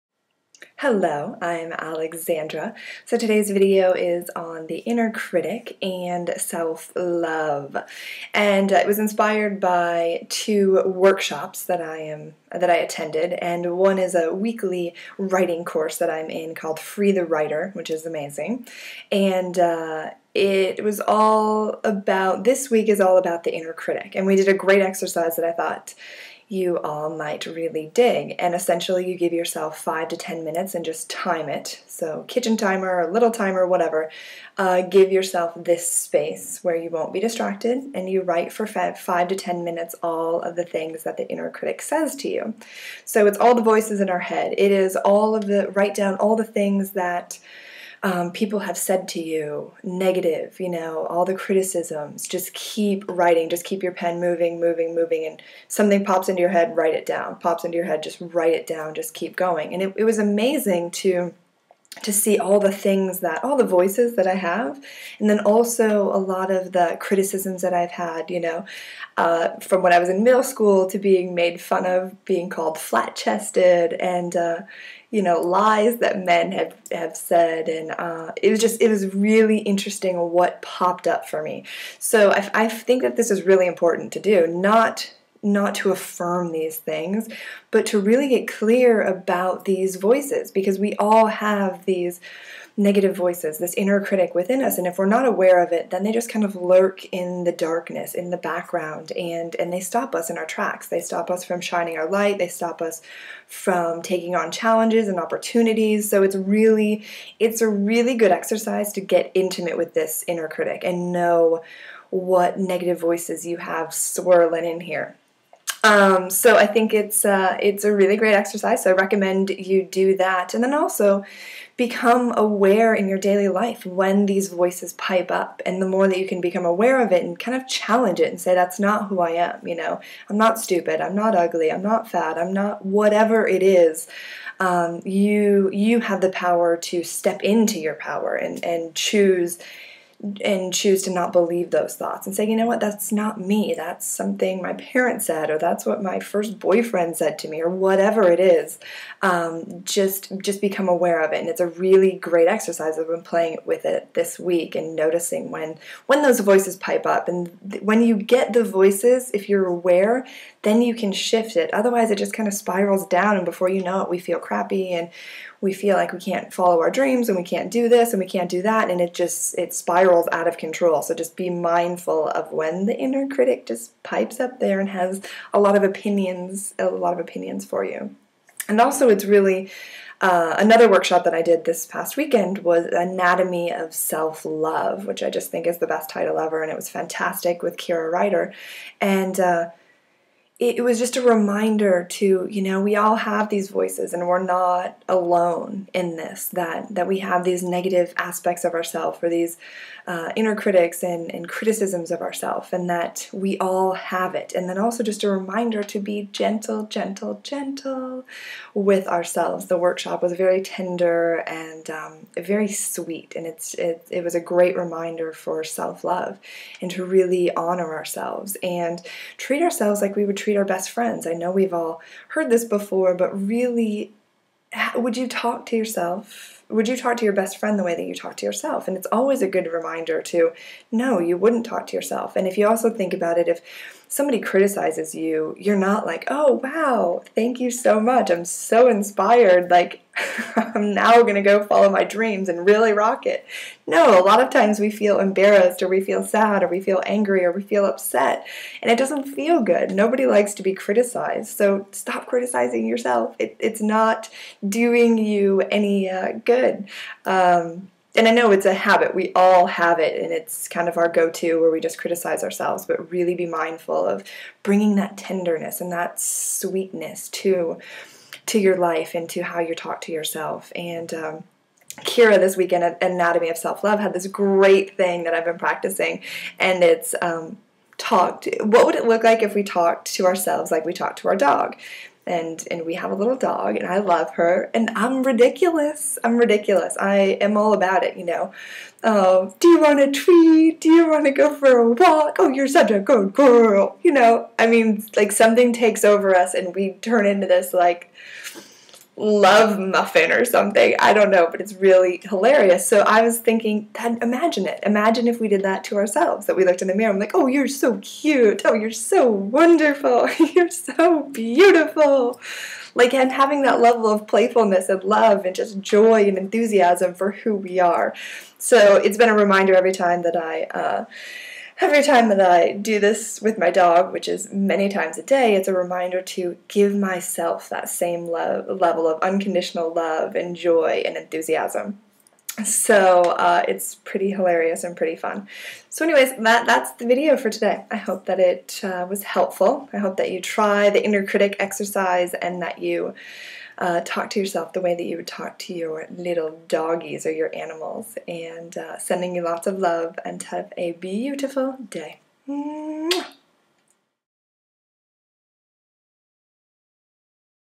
Inner Critic & Self Love
This video is about a way to detect the inner critic and how to love yourself up even more! :)